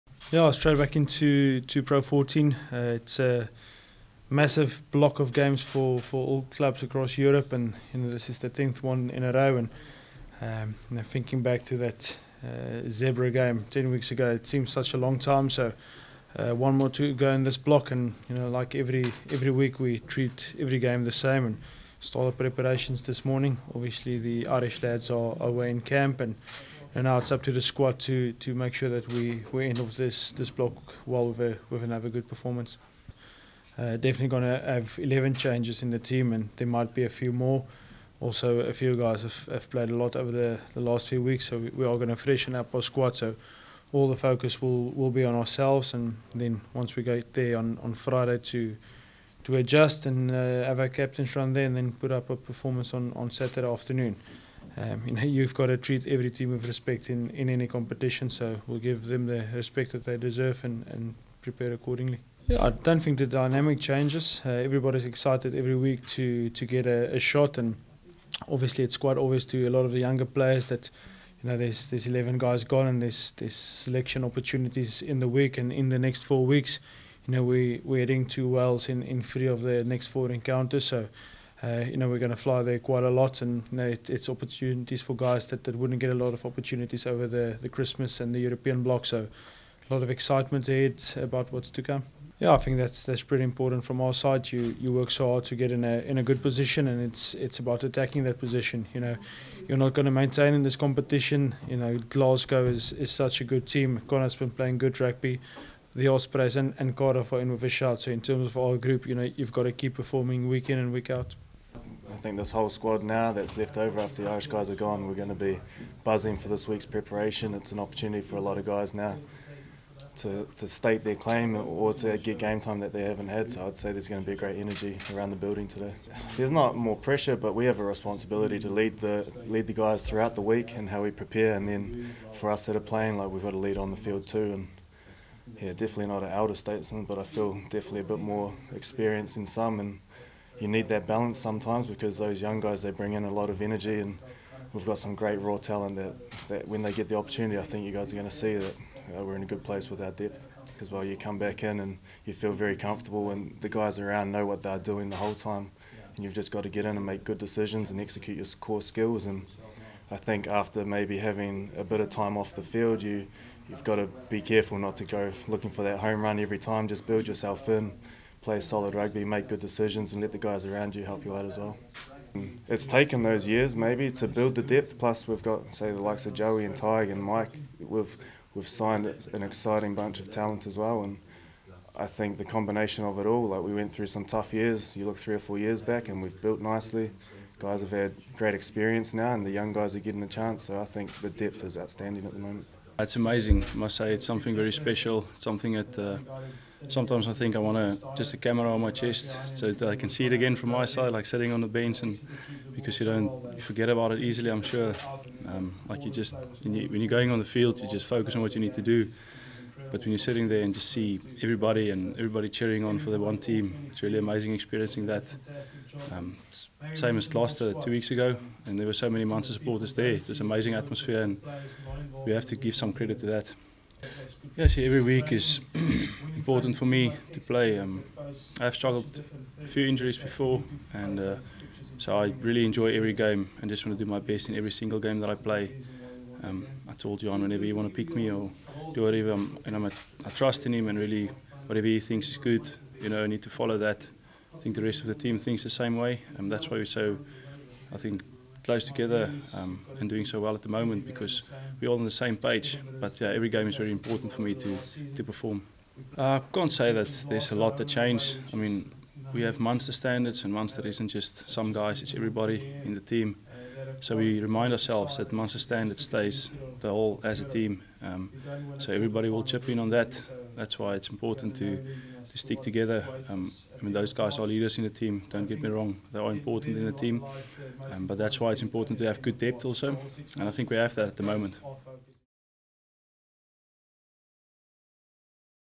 Press Pass | Dragons v Munster
Head Coach Johann van Graan, Tyler Bleyendaal and Arno Botha look ahead to Saturday's Guinness PRO14 clash against Dragons at Rodney Parade (3pm).